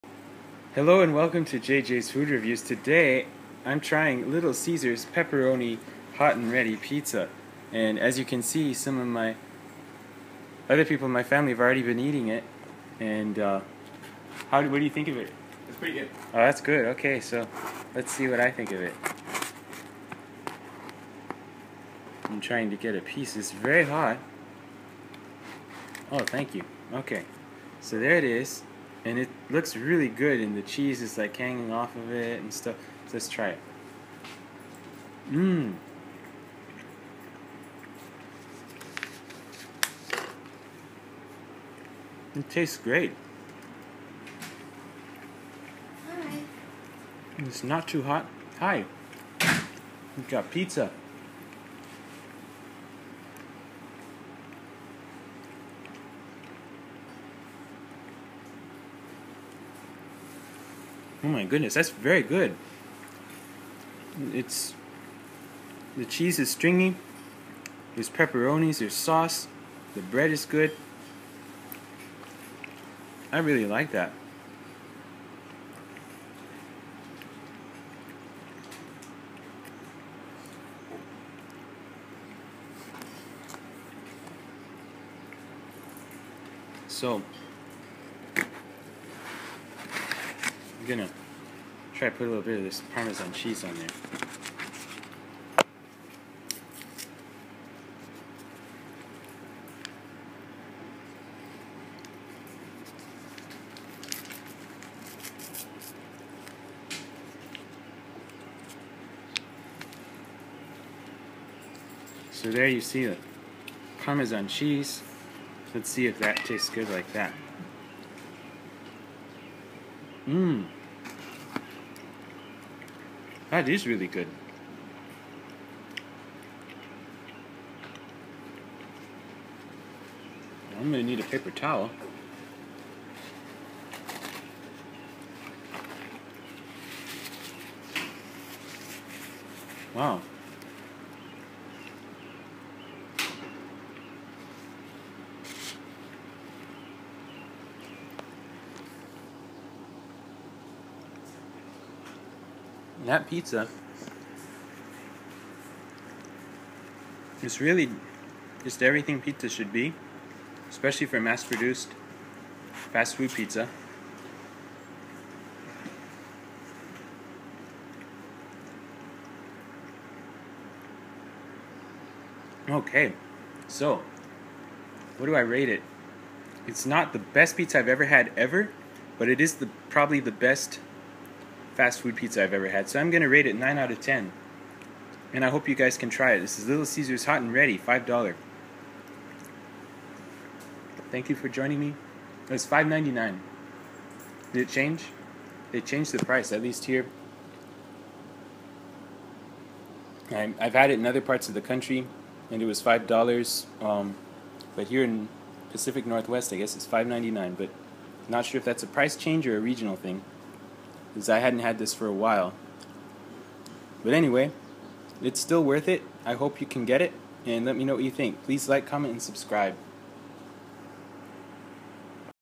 Little Caesars Hot-N-Ready Pepperoni Pizza
For supper, we got some Little Caesars Pizza - and it was a hit. In today's episode, I give my rating and share some thoughts about this pizza.